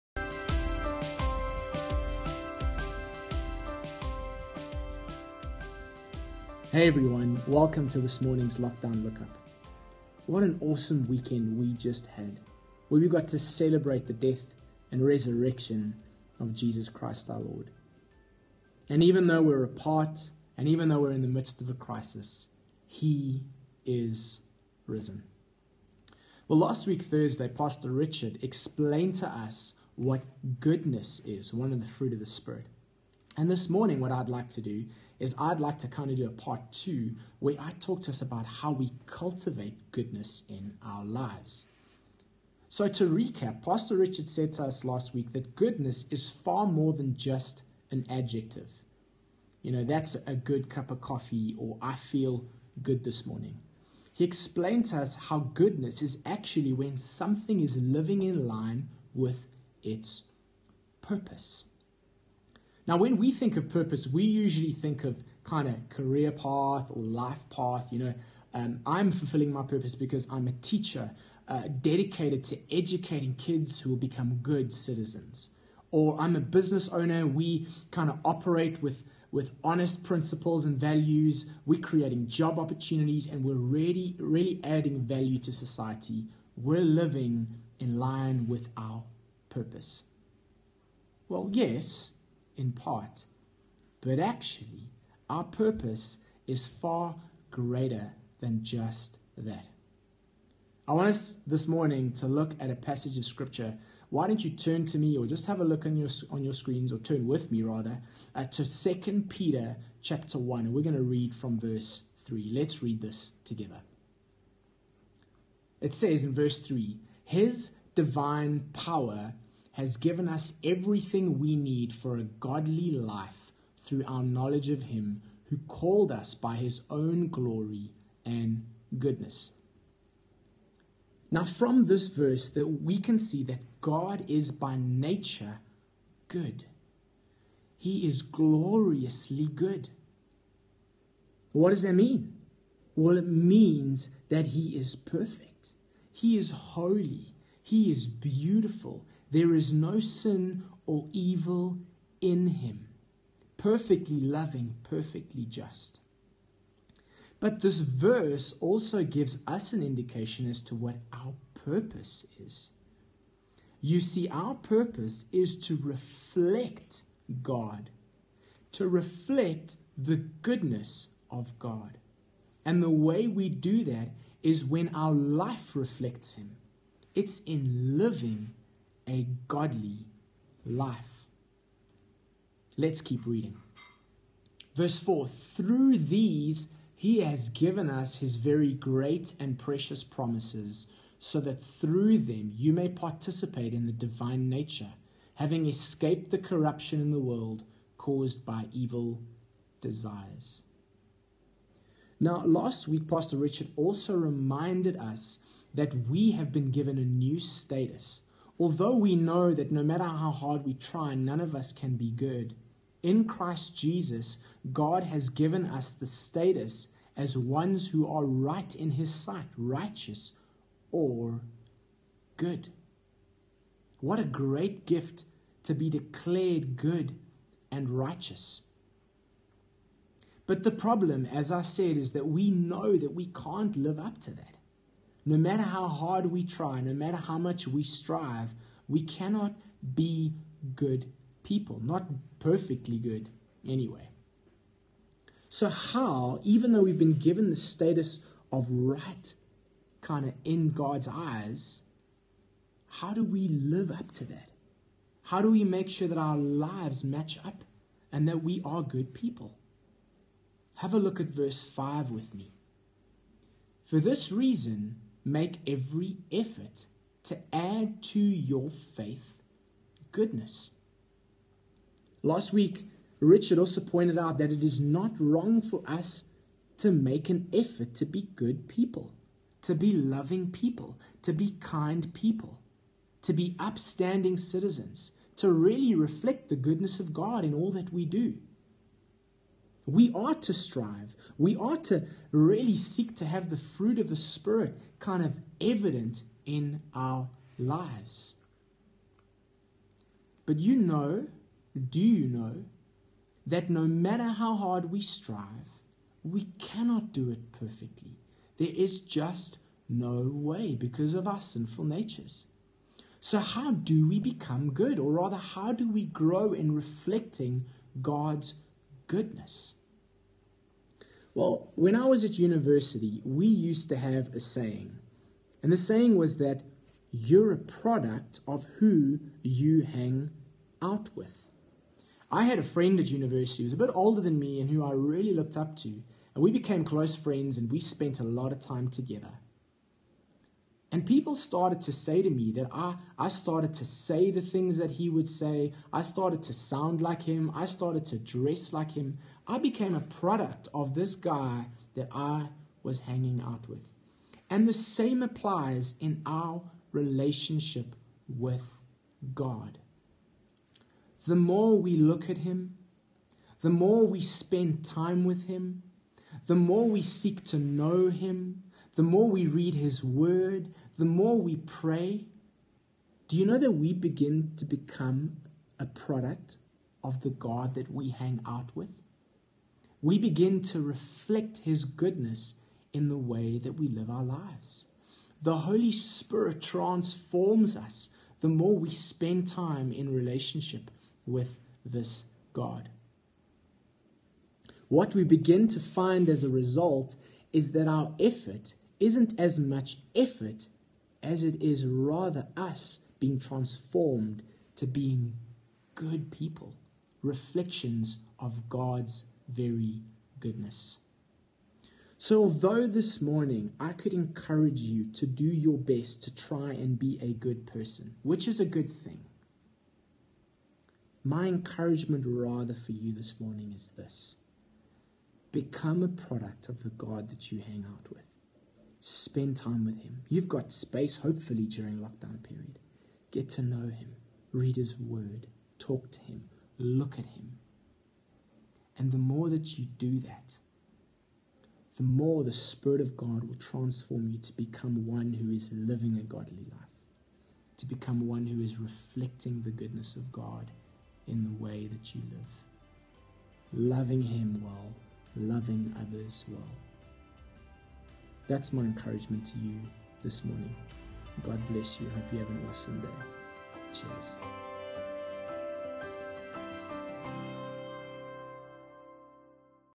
Lockdown Lookup - 13 April
Goodness (part 2)
Dave Myburgh shares some reflections on how to cultivate the fruit of Goodness from 2 Peter 1:3-5